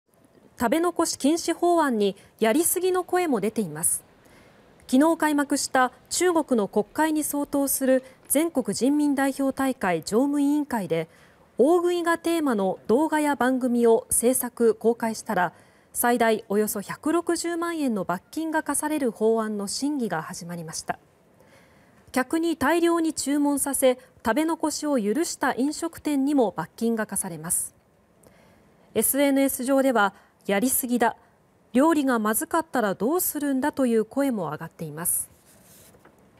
中国「食べ残し禁止」法案 最大約160万円の罰金も(2020年12月23日)
「食べ残し禁止」法案に「やり過ぎ」の声も出ています。

　22日に開幕した中国の国会に相当する全国人民代表大会常務委員会で「大食い」がテーマの動画や番組を制作・公開したら最大約160万円の罰金が科される法案の審議が始まりました。客に大量に注文させ、食べ残しを許した飲食店にも罰金が科されます。SNS上では「やり過ぎだ。料理がまずかったらどうするんだ」という声も上がっています。習近平国家主席が8月に取り締まりを強化する指示を出したことで、中国の若者に人気だった「大食い映像」はSNSから消えていました。